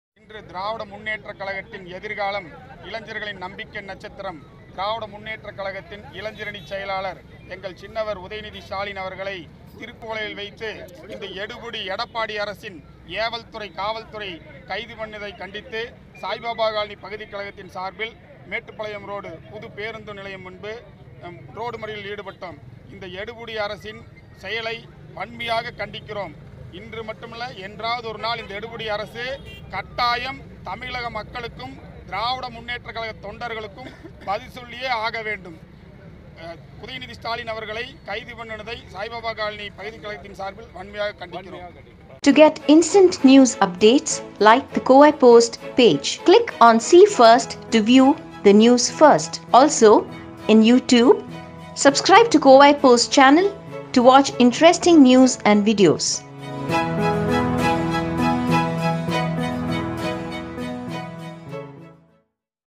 கோவையில் திமுகவினர் சாலை மறியல்
உதயநிதி ஸ்டாலின் கைது செய்யப்பட்டதை கண்டித்து கோவை சாய்பாபா காலனியில் திமுகவினர் சாலை மறியலில் ஈடுபட்டனர்